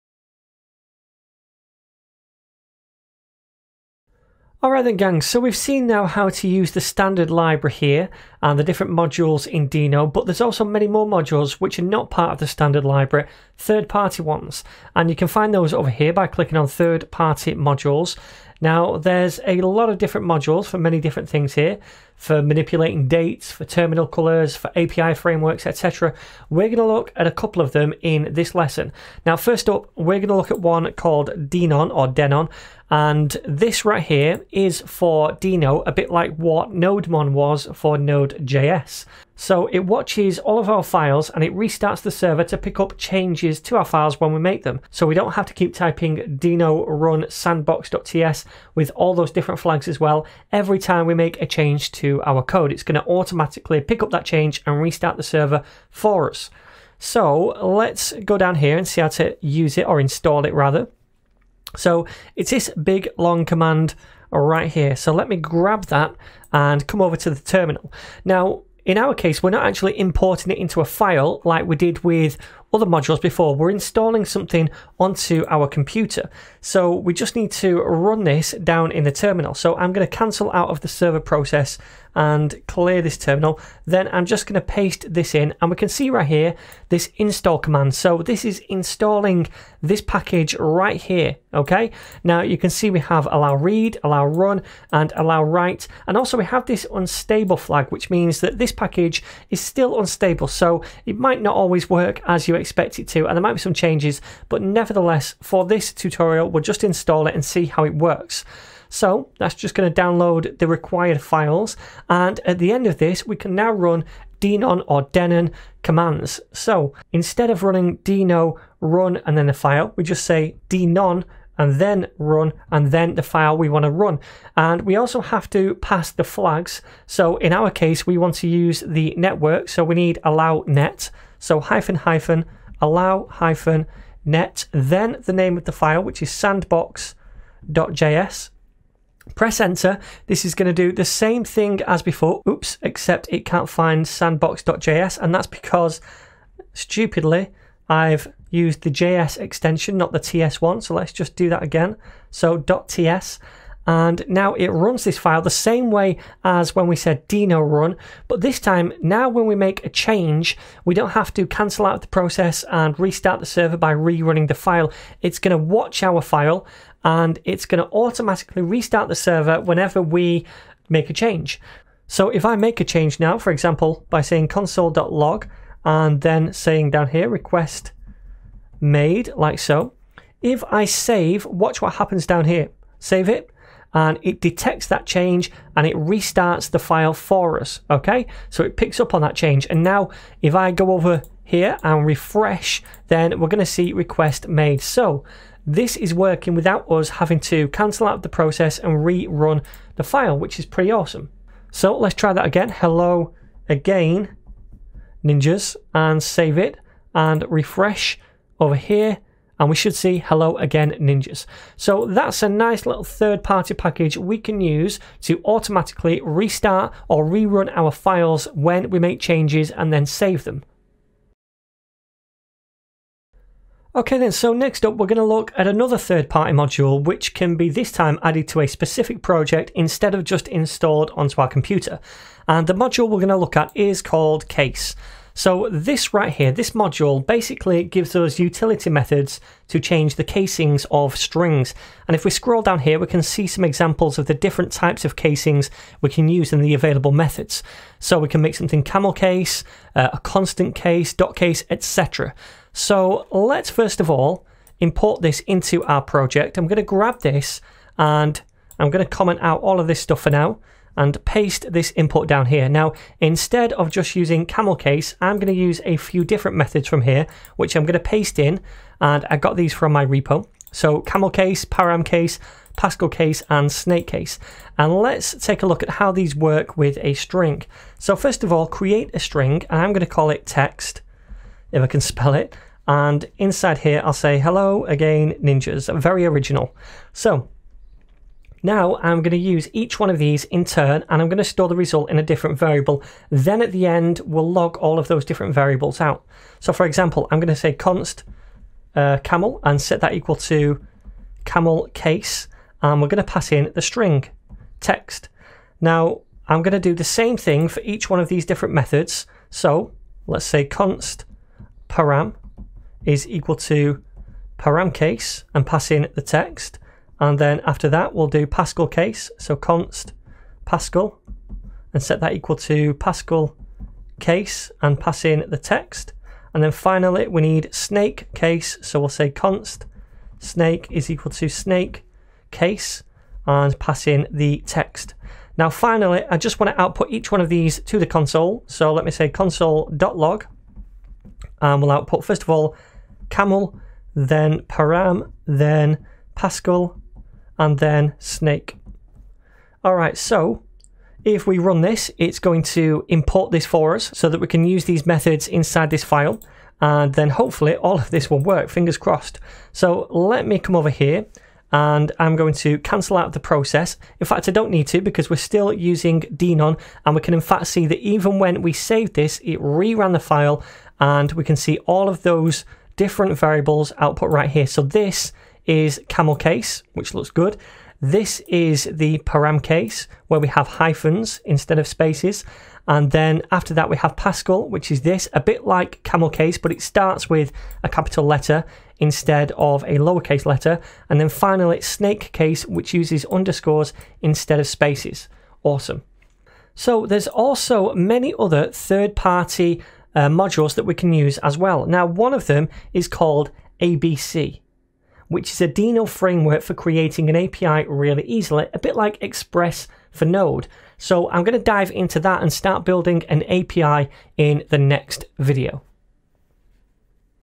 Deno Tutorial #4 - 3rd Party Modules
Hey gang, in this deno tutorial we'll take a look at a few of te 3rd-part modules we have.

--- Chapters ---
0.00 - denon
3:42 - the 'case' module